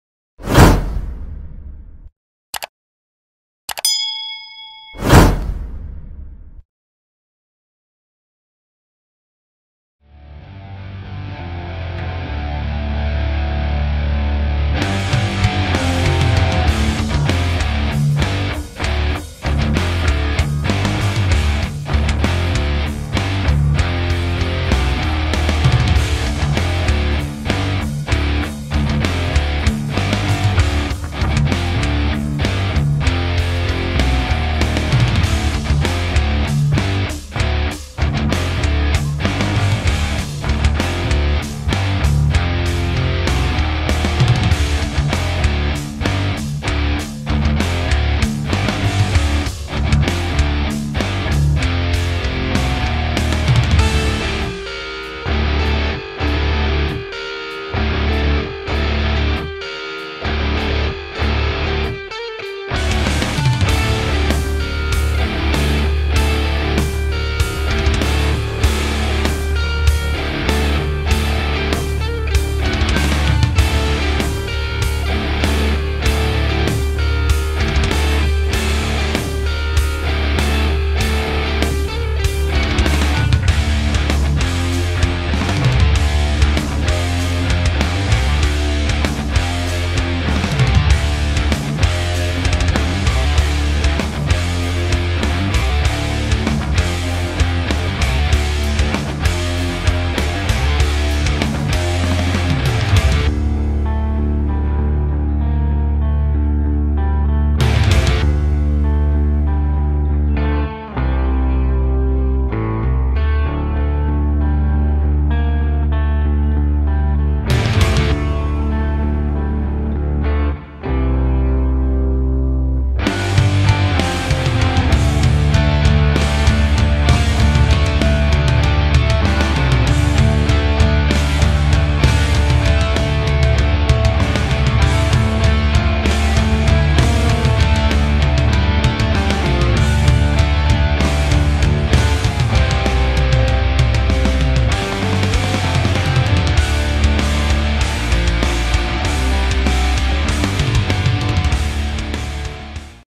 automotive photography on a Australian muscle car Ford
Here is a slide show of a photo shoot I did a couple of days ago.  If you would like to see more of my image's there is a link below to my like Facebook page and Instagram where I post all my image's.